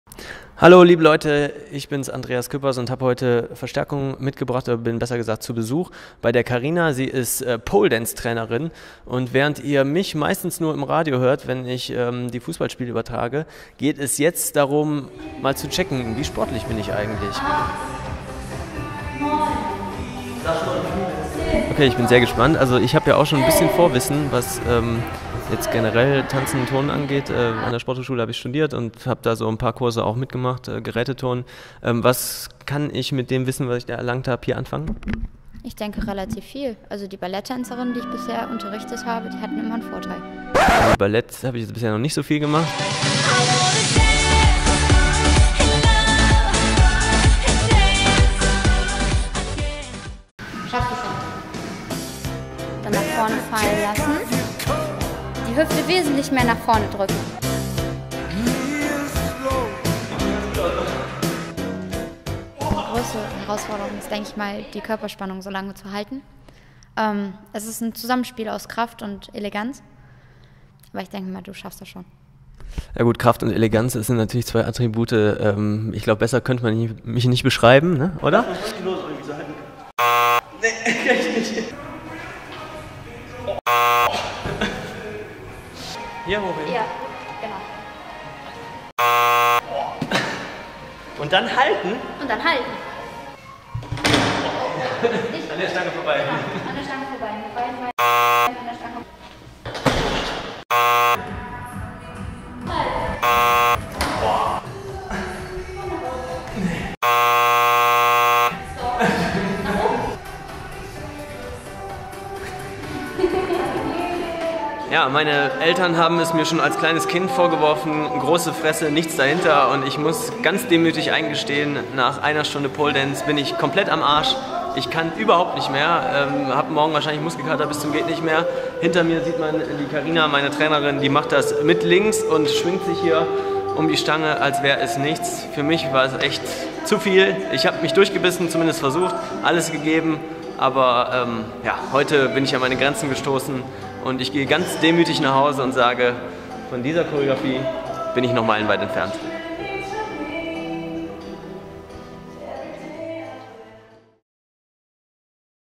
Andreas Cüppers beim Pole-Dancing
Wie sportlich ist unser Sportreporter? Andreas Cüppers liefert eine Woche lang die Antwort! Los geht's beim Pole-Dancing in der Tanzschule More than Dancing in Mönchengladbach.